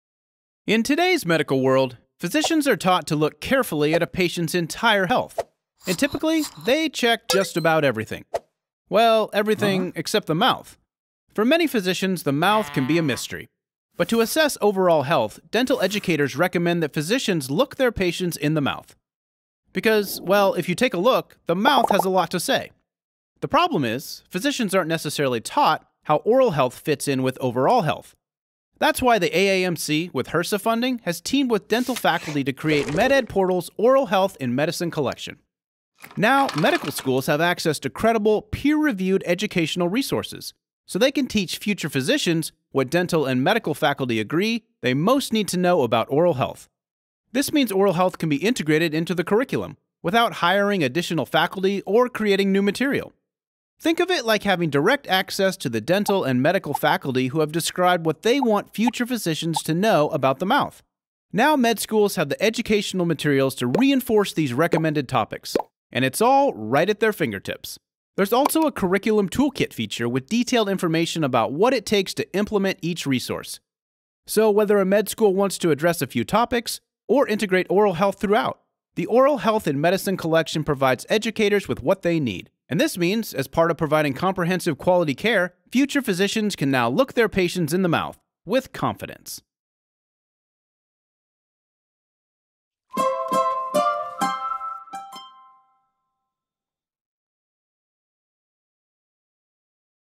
Learn About the Oral Health in Medicine Collection
Video Shorts Featuring Oral Health in Medicine

Three 90 second animated video shorts have been created to help frame oral health in medicine.  These can be used as part of a shared learning experience or independently for asynchronous learning, either for faculty or as pre-engagement preparation for learners.

Support for the oral health in medicine video shorts was sponsored in part by funding from the Health Resources and Services Administration/Maternal Child Health Bureau grant U44MC20223.